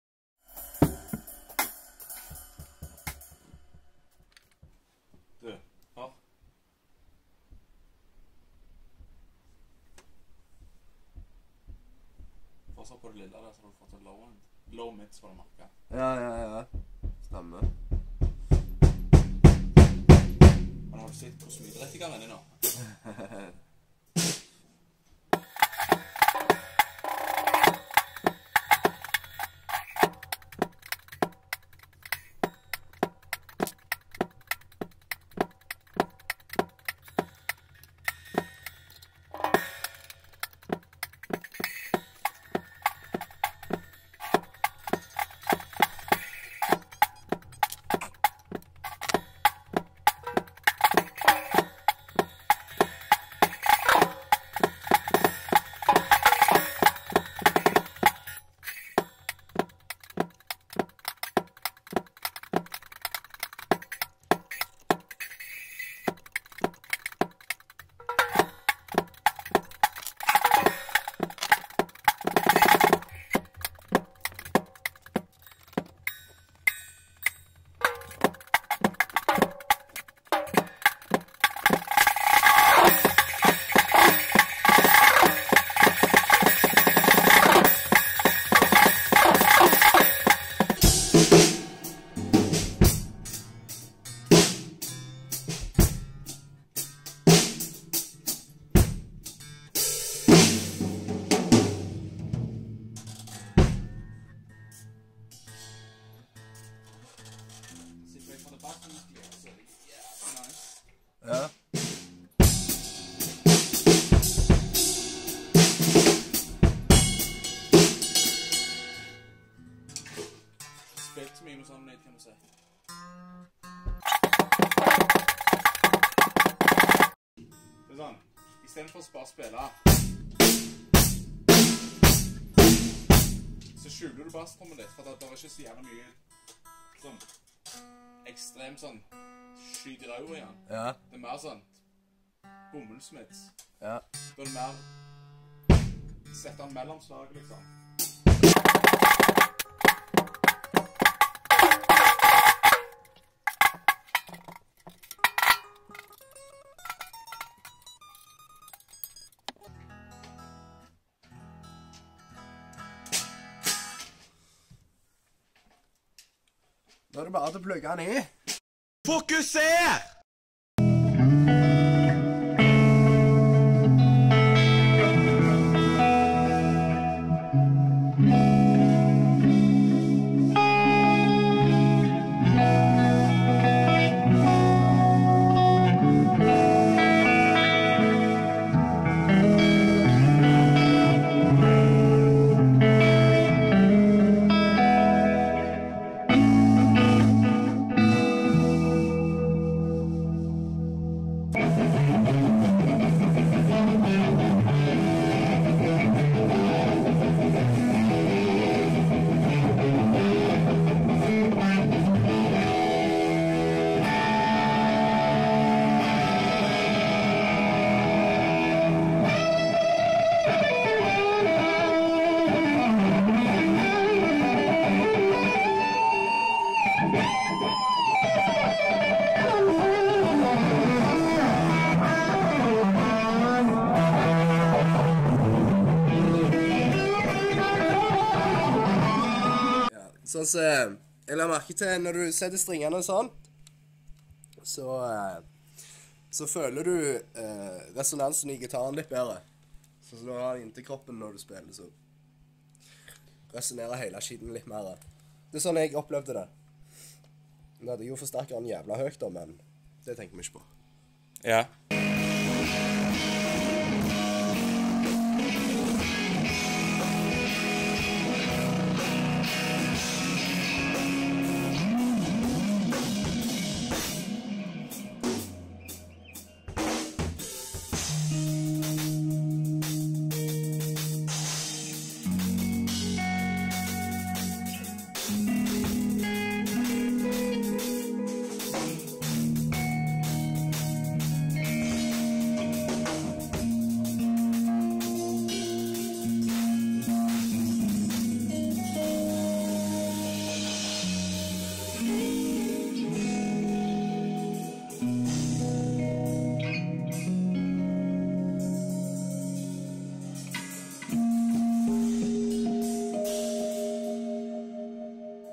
Changing strings on my Gibson
litt av prossesen i strengeskifte, og e forklare litt om ein litt anderledes måte å re-string'e ein Gibson, mens Tory lefser på trommene.